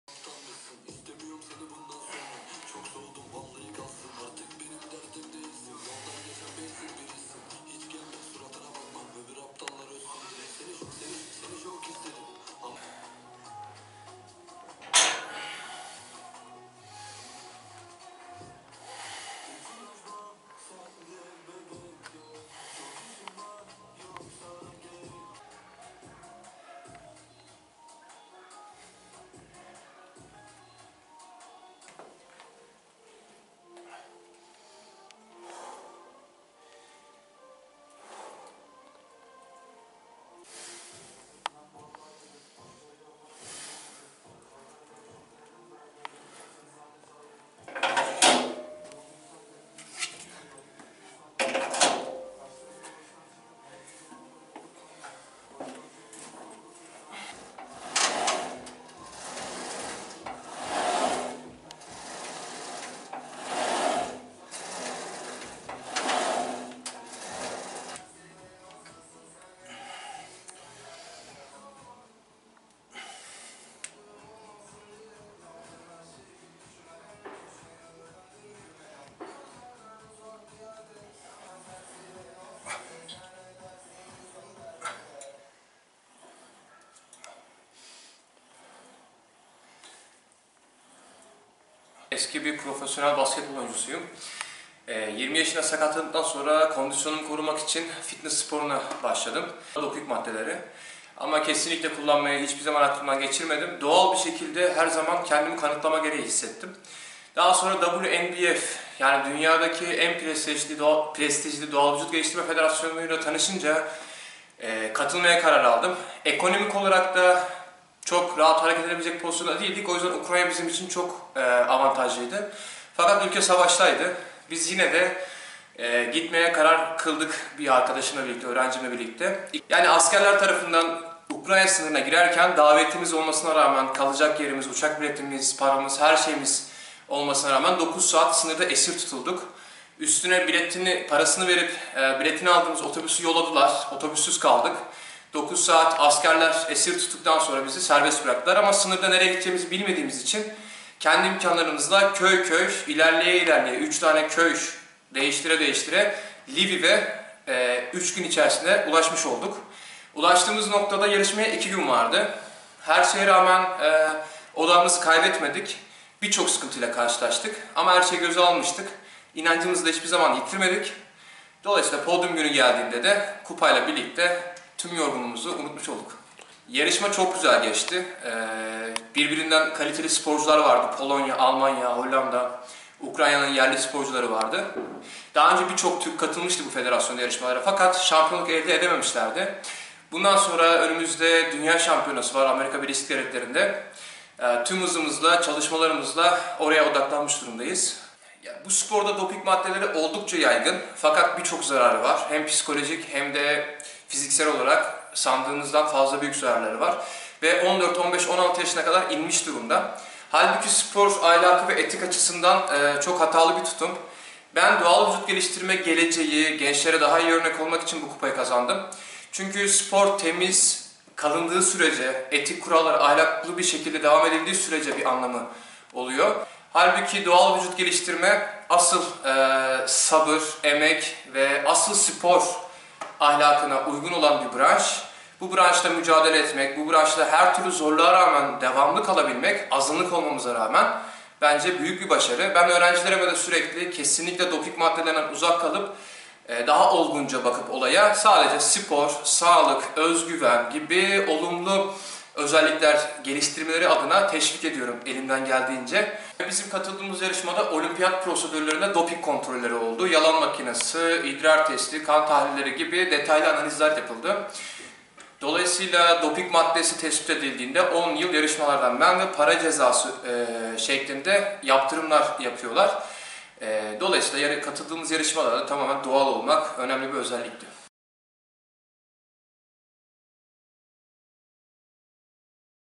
EREĞLİ'DEN ÇIKIP SAVAŞ BÖLGESİNDE AVRUPA ŞAMPİYONU OLDU SIRADA DÜNYA ŞAMPİYONLUĞU VAR
- Karadeniz Ereğli'den savaş bölgesi Ukrayna'ya gidip 9 saat rehin tutulduktan sonra salıverilen ve 3 gün sonra yarışmanın yapılacağı salona giderek Avrupa Doğal Vücut Geliştirme Şampiyonu olan 29 yaşındaki Ömer Faruk Darçın, Kasım ayında gerçekleştirilecek Dünya Şampiyonluğuna hazırlanıyor.

- Destek aramak için gittiği Karadeniz Ereğli Belediyesi'nden, "sadece futbola destek veriyoruz" diyerek gönderilen Amerika'da yapılacak şampiyonaya da kendi imkanları ile hazırlanan genç sporcu Türkiye'ye altın madalya getirmek hedefinde.